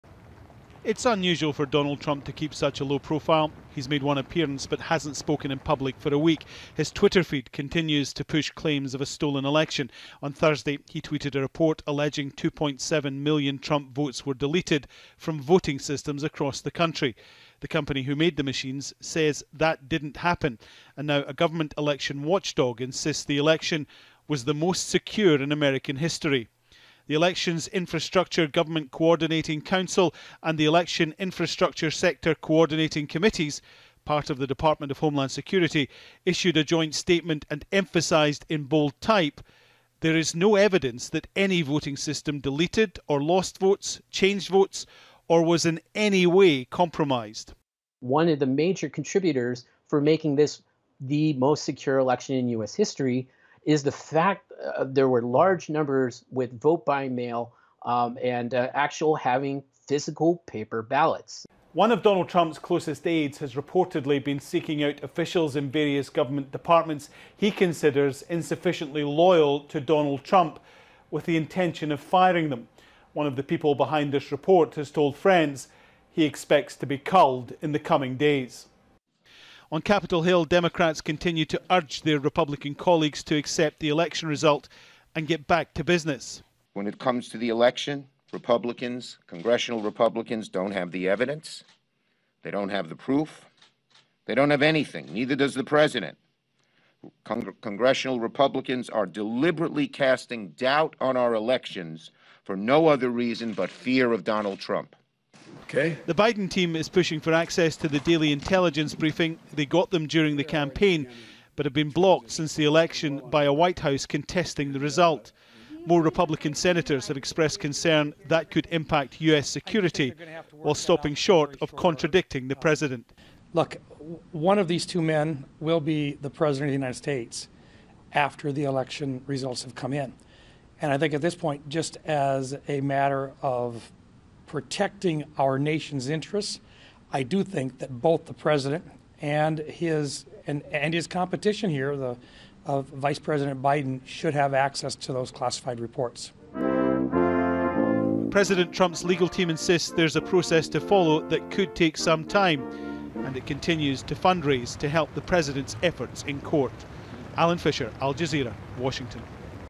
‘No evidence’: US election officials reject Trump claims of fraud
Security and election officials from the US government have released a statement declaring this year's presidential election was the 'most secure in American history'.
They say there is no evidence voting systems were compromised, as President Donald Trump continues to make unsubstantiated claims of widespread voter fraud.
Al Jazeera's Alan Fisher reports.